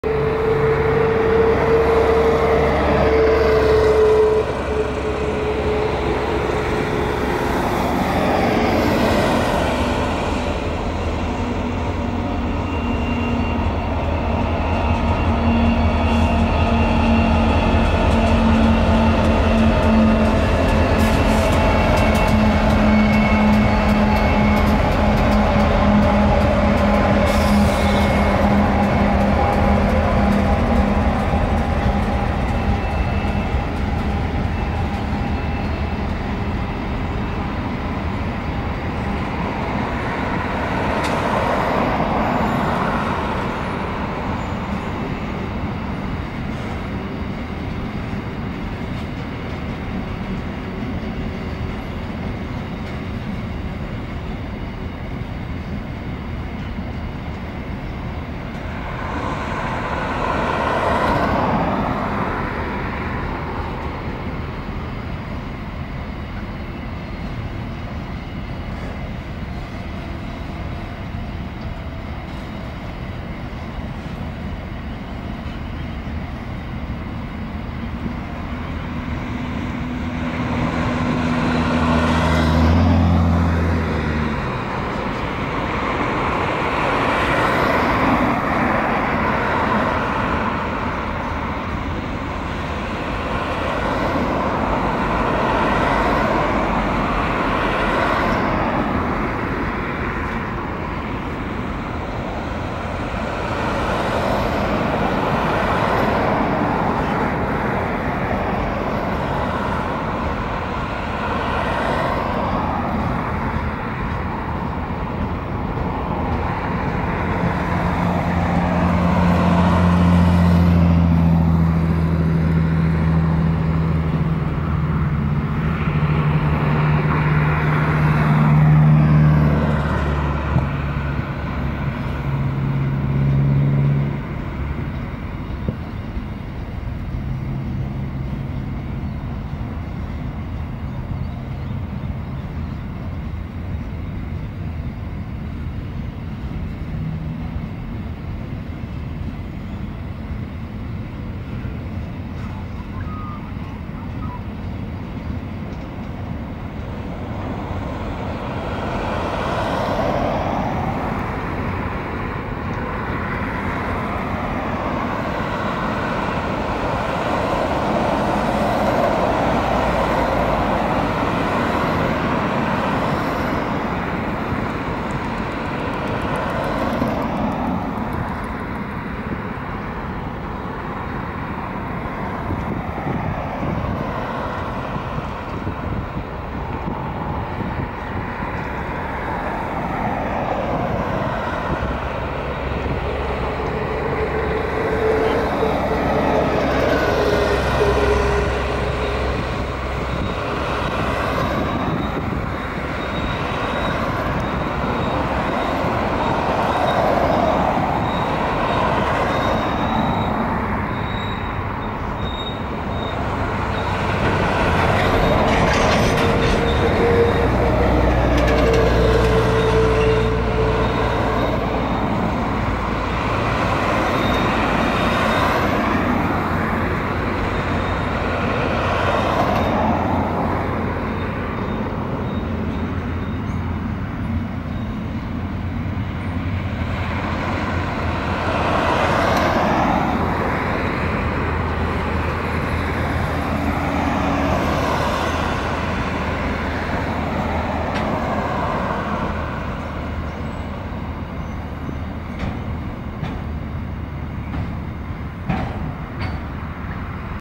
C505 / PHC002 / PHC001 / EL60 with Crawford's 1547 - 24/7/20
C505 / PHC002 / PHC001 / EL60 crawl by leading Crawford's / SSR service 1547 from Port Botany/Cooks River to Werris Creek NSW.

It is closely behind an all stations to Newcastle service so it is receving amber signals.

Seen at Gosford on Friday 24th July 2020 at 15:24